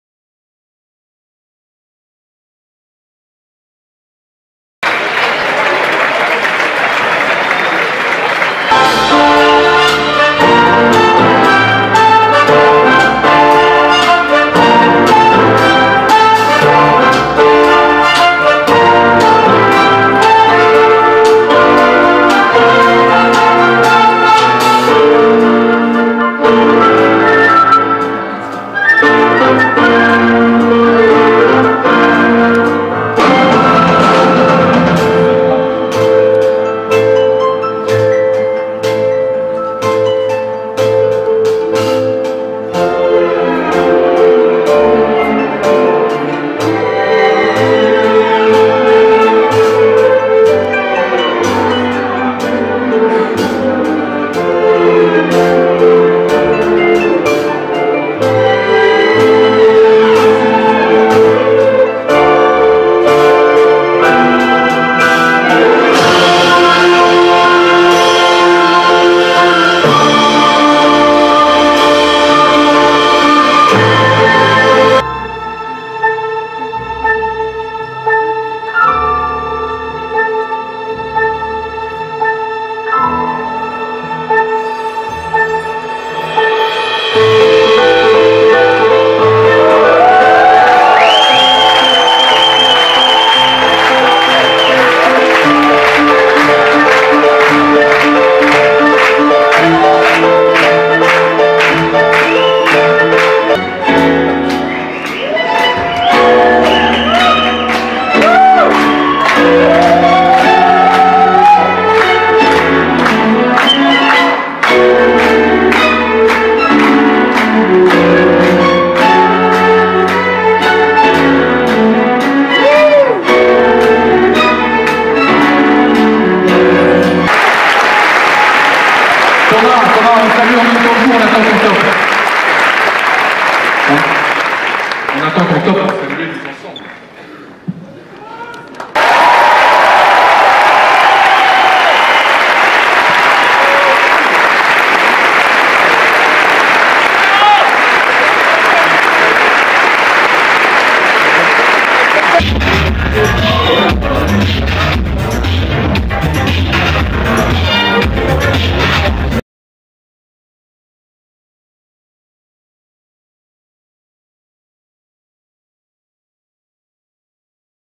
Ed Banger 15 ans symphonique au Grand Rex
le grand concert anniversaire du label Ed Banger. l orchestre Lamoureux sous la direction du chef Thomas Roussel interprète les succés du label. Pedro Winter,  Justice, DJ Mehdi, Breakbot, Cassius, m OIZO, 10lec6, SoMe. la soirée etait sold out et l'after au ReX Club a permis aux fans de venir écouter leurs idoles.